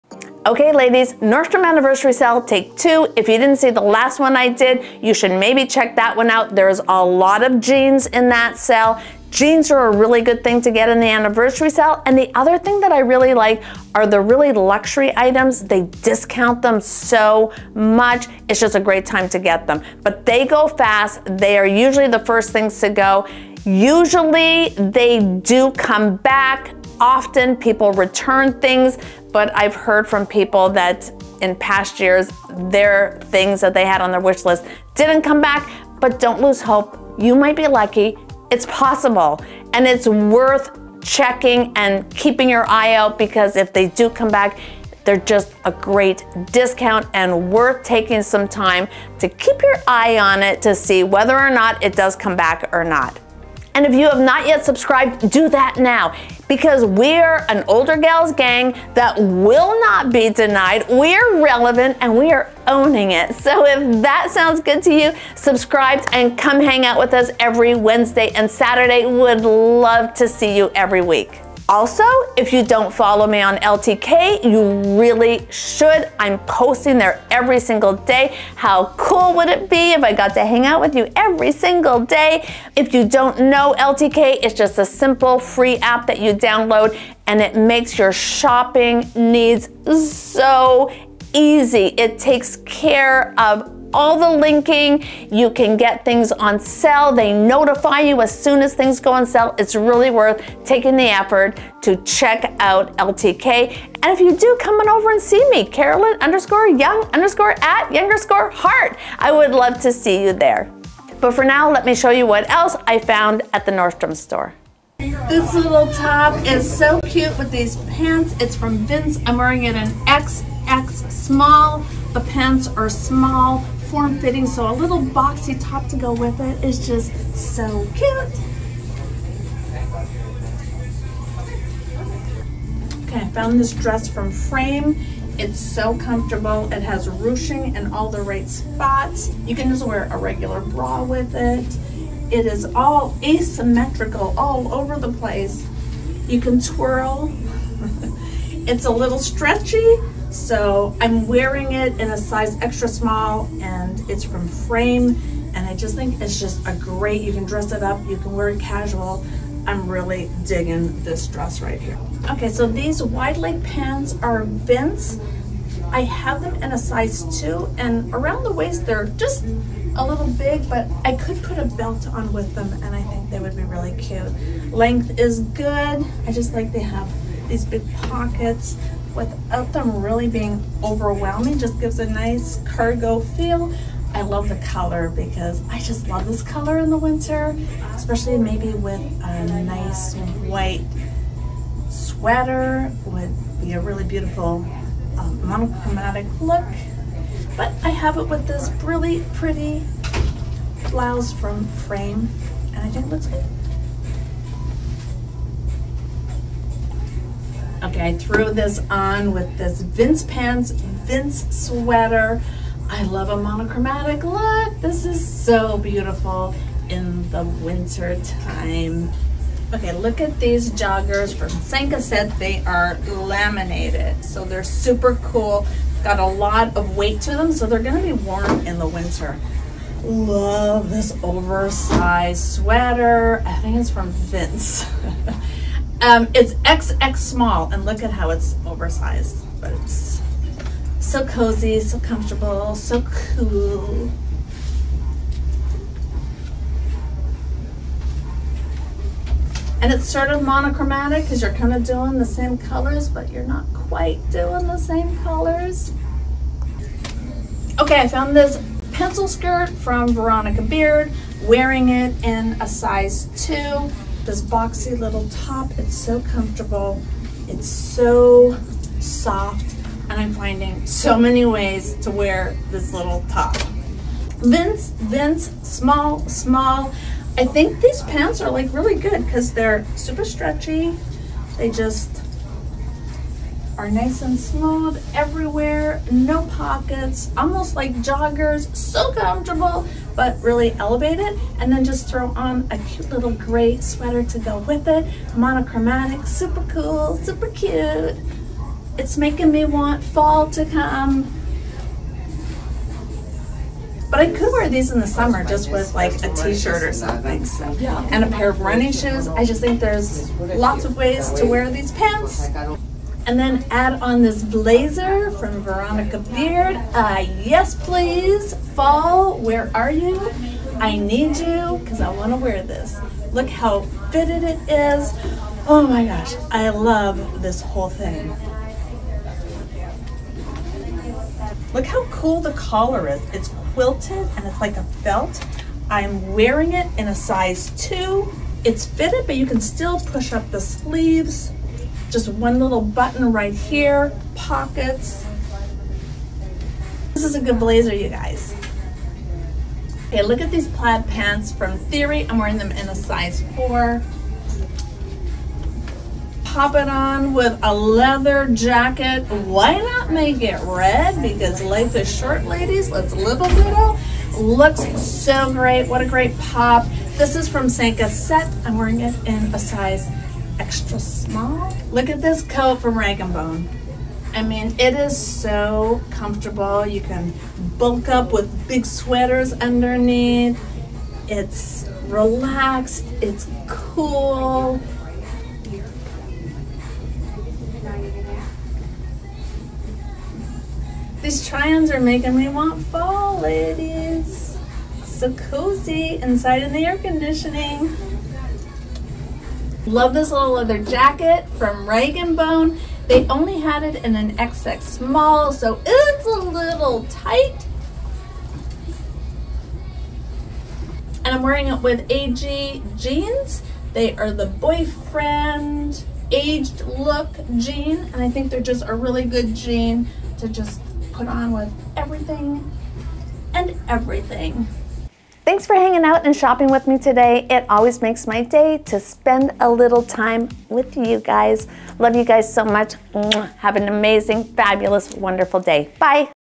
So Many Great Choices! You Are NOT Going to Want to Miss Out!!
Videos if you're SICK of hearing about the Nordstrom Anniversary Sale!!

The Nordstrom Anniversary Sale is full on and it's time to start shopping. If your date to shop doesn't start until July 15 then you still have time to fill you cart and be ready! Things that have sold our from the first round will start to come back and you need to be diligent on keeping you eye out for sold out items to get returned.

Vince Boxy white top
Vince white sweater
Frame silk blouse
Vince leggings
Frame Dress
Vince White cotton twill cargo
cinq a sept cargo jogger
oversized cardigan
Veronica Beard Blazer
Rag and Bone Coat
Veronica Beard Croc embossed leather Skirt
Vince short sleeve grey sweater
Cinq a Sept faux leather RED jacket
Theory houndstooth pant
AG exboyfriend Jean
Rag and Bone Brown leather jacket